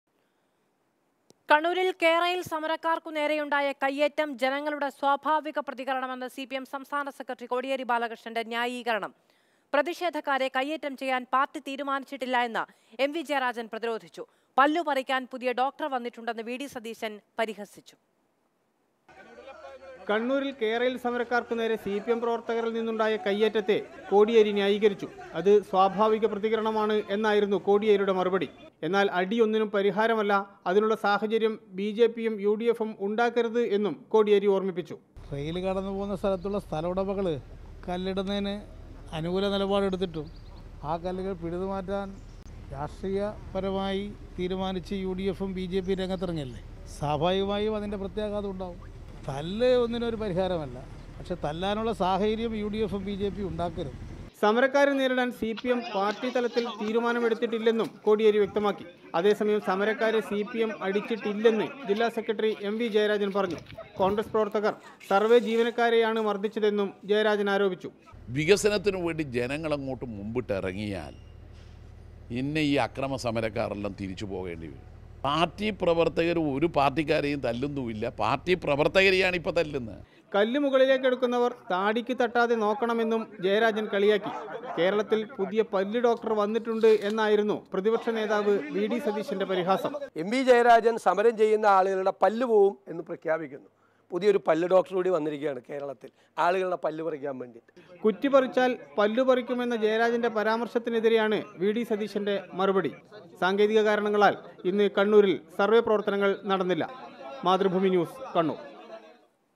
സമരക്കാർക്ക് നേരെയുണ്ടായ കൈയേറ്റം ജനങ്ങളുടെ സ്വാഭാവിക പ്രതികരണം: കോടിയേരി | Mathrubhumi News
കണ്ണൂരിൽ കെ-റെയിൽ സമരക്കാർക്ക് നേരെയുണ്ടായ കൈയേറ്റം ജനങ്ങളുടെ സ്വാഭാവിക പ്രതികരണമെന്ന്സിപിഎം സംസ്ഥാന സെക്രട്ടറി കോടിയേരി ബാലകൃഷ്ണന്റെന്യായീകരണം

Happy viewing!